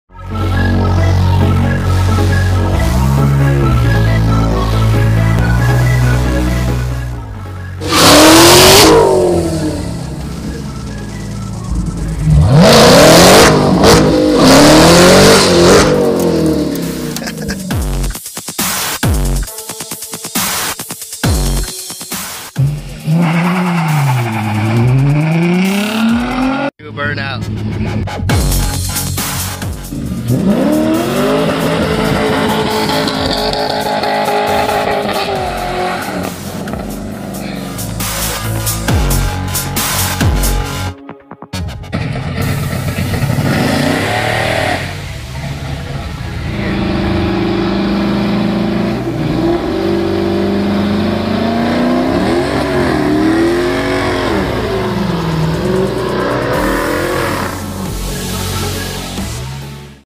Spinning Tires & Whipping Donuts at LIS Audio
LIS Audio Friends and Cleints having a bit of fun with their cars! Every now and then someone likes to roast so rubber on their way out. We don't mind lighting up our private drive every so often.

Cars in the line-up...
2017 Dodge Hellcat SRT
2001 Subaru Legacy Impreza
2006 Infiniti G35
2005 Chevrolet Corvette
1962 Ford F100
2008 Dodge Charger RT

You can read more in depth about other project on our LIS Audio blogs: